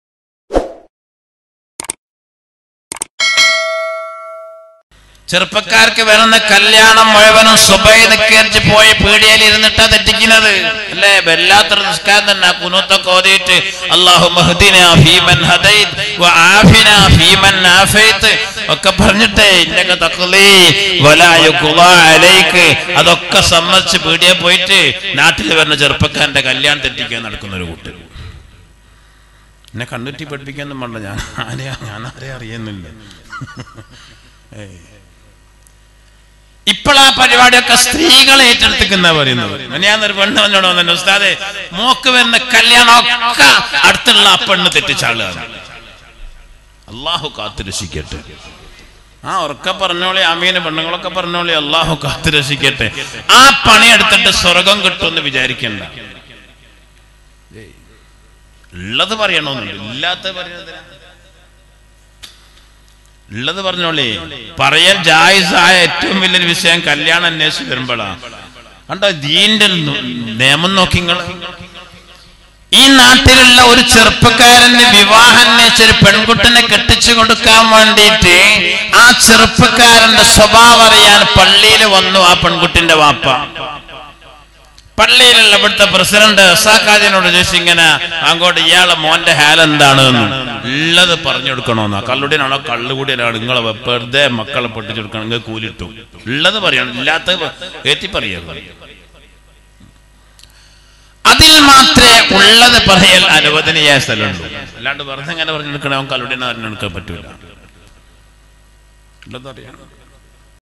കല്യാണ കാര്യത്തിൽ ഇദ് സൂക്ഷിക്കുക | Malayalam Islamic Speech
Speaker: Majeed Baqavi
Program: Islamic Speech Malayalam
Place: Kerala

all videos are copyrighted by viwo media. so please dont use videos without permission